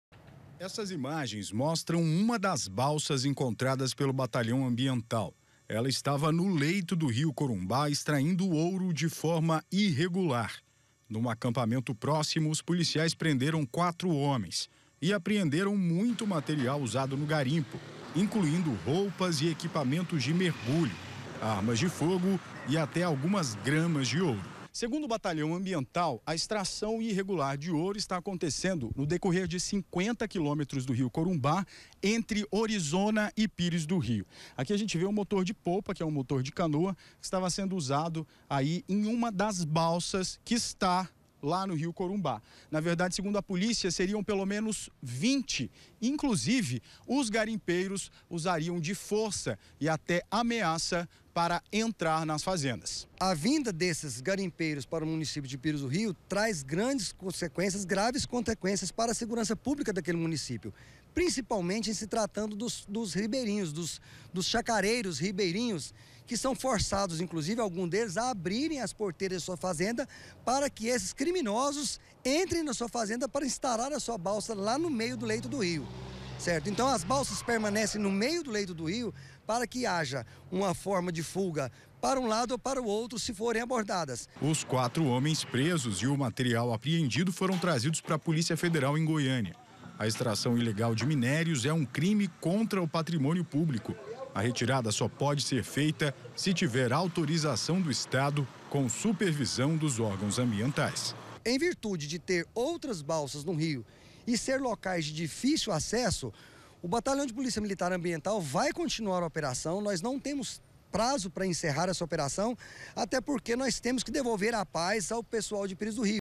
CRIMINOSOS SÃO PRESOS EM GARIMPO ILEGAL DE EXTRAÇÃO DE OURO
A polícia descobriu um garimpo de extração ilegal de ouro, em Pires do Rio. Quatro homens foram presos e materiais apreendidos. Segundo moradores da região, os criminosos ameaçavam os donos das fazendas para invadir o local e extrair o ouro.